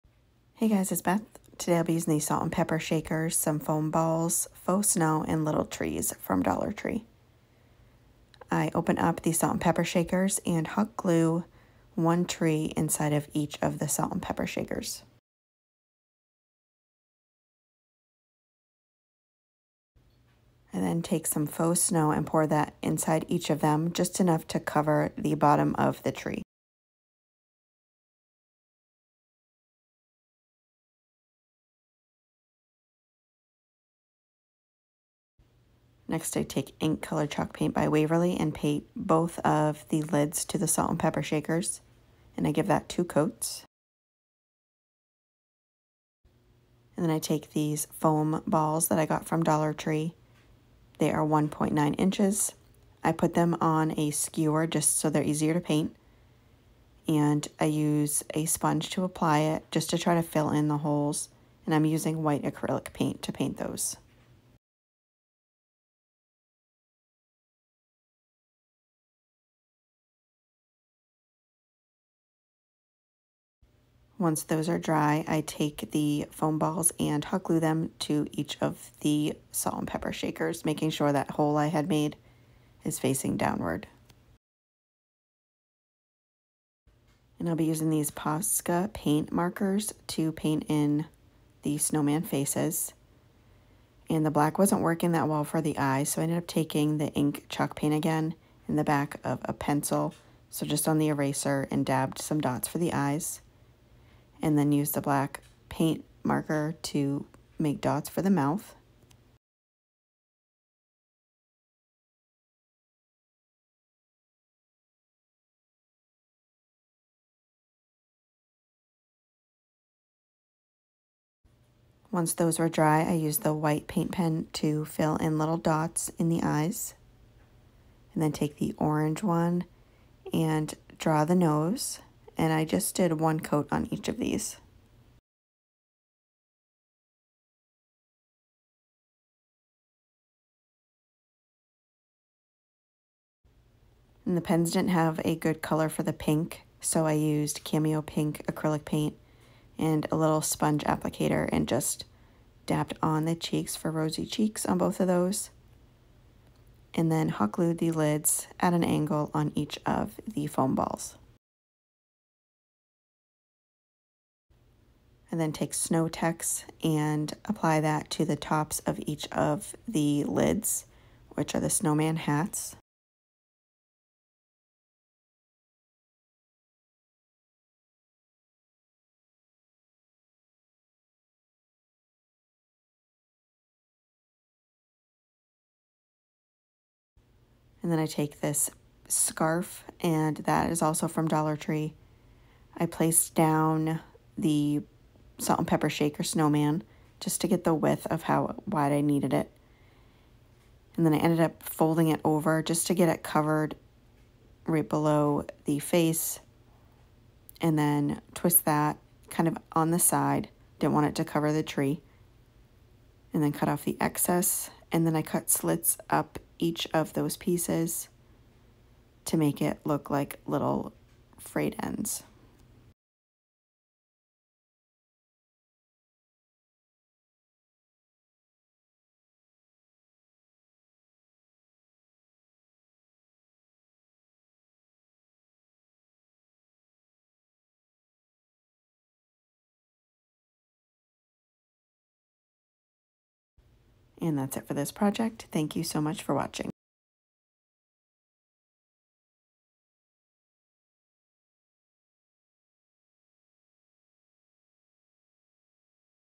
Salt & Pepper Shaker Snowmen DIY || Christmas DIY || Just 1 Easy Craft
I was inspires to make these by Debbie at Refresh Restyle Crafts & DIY. I thought they were so adorable and had to make some! I hope they inspire you too!

Materials~

Dollar Tree~
Salt & Pepper shakers
1.9” foam balls
Faux snow
Small Christmas trees
Scarf

Hobby Lobby~
Snow Tex

Other~
Ivory & Ink chalk paint ~ Waverly
Cameo pink acrylic paint
Paint pens - I used Posca brand
Hot glue